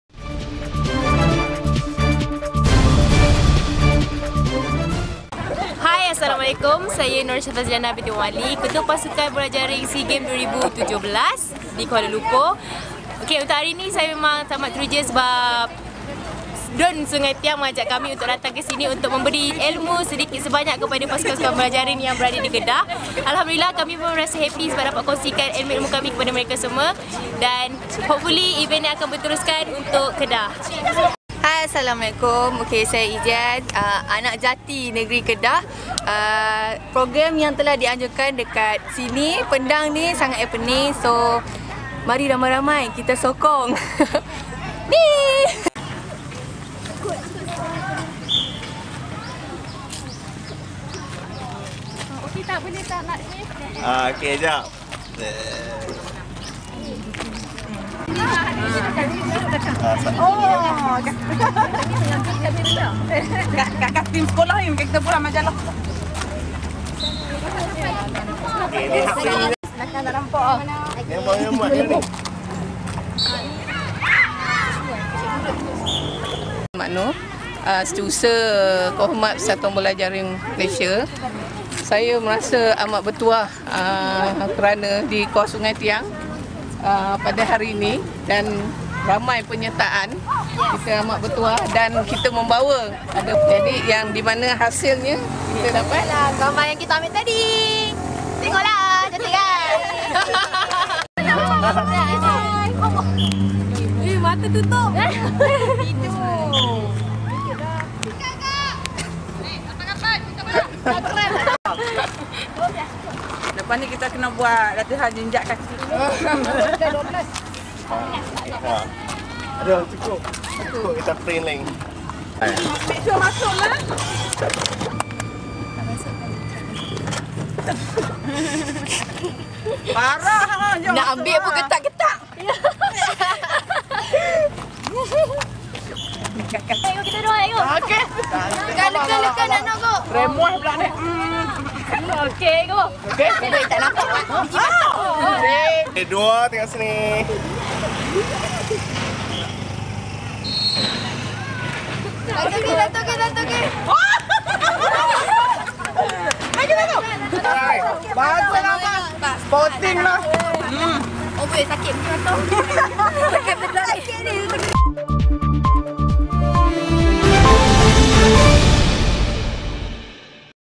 KARNIVAL BOLA JARING BERSAMA BN DUN SUNGAI TIANG, DATUK SURAYA YAACOB
Calon BN DUN Sungai Tiang, Datuk Suraya Yaacob mengambil tindakan kreatif dengan meletakkan Photo Booth secara percuma kepada peserta Karnival Bola Jaring Bersama Pasukan Malaysia di Sekolah Menengah Kebangsaan Sungai Tiang bagi membolehkan mereka merakam gambar sebagai kenang-kenangan.

Ketua bola jaring pasukan Malaysia Sea Games 2012, Norshifa Ziana Mohamad Ali berkata, dia begitu teruja di jemput DUN Sungai Tiang untuk menyertai karnival bola jaring dan dapat mencurahkan segala pengalaman kepada peserta-peserta yang menyertai karnival tersebut,.

Selain itu DUN Sungai Tiang juga menyediakan Photo Booth bagi membolehkan peserta merakam gambar.

Setiausaha Kerhormat Persatuan Bola Jaring Malaysia,Zainon Md Noor berkata, dia berasa amat bertuah kerana dijemput calon BN DUN Sungai Tiang, Datuk Suraya Yaacob ke Karnival Bola Jaring Bersama Pasukan Malaysia dan dia membawa bersama empat pemain kebangsaan menyertai karnival tersebut bagi membolehkan mereka berkongsi pengalaman dengan peserta lain.

Karnival Bola Jaring Bersama Pasukan Malaysia yang bermula jam 8.00pagi dan berakhir pada jam 5.00petang.